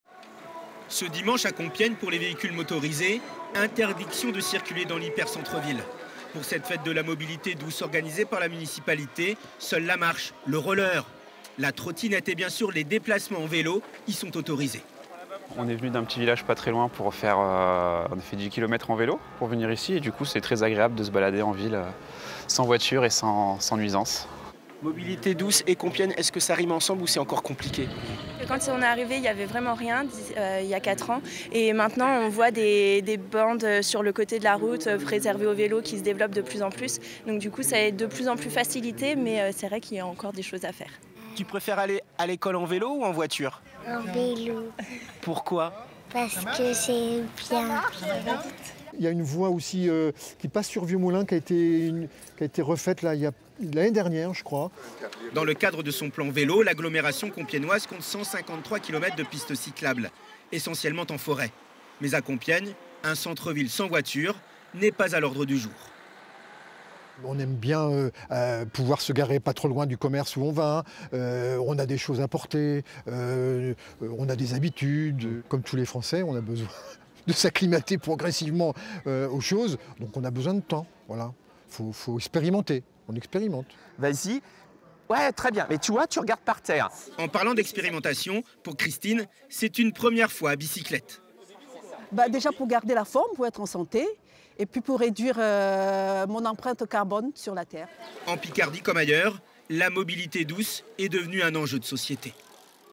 Compiègne expérimente la mobilité douce et interdit la circulation des voitures pendant une journée
Pendant une journée, la circulation des véhicules motorisés a été interdite dans le centre-ville de Compiègne. La ville essaie de sensibiliser aux mobilités douces : le roller, le vélo ou encore la trottinette. Une journée pour peut-être modifier notre vie quotidienne.